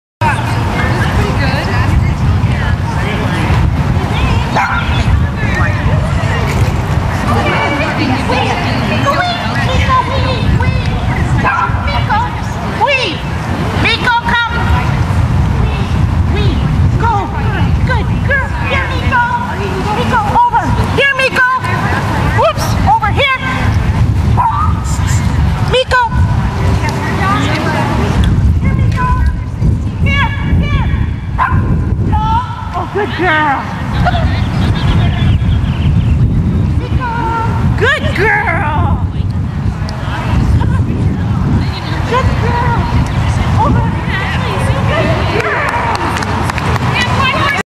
Alyce and Miko Novice Jumpers 3-30-13
Alyce and Miko, Basenji, run an AKC Novice Jumpers course under Judge Alan Arthur at the Agility Club of San Diego agility trial at NTC Promenade, San Diego,  3/30/2013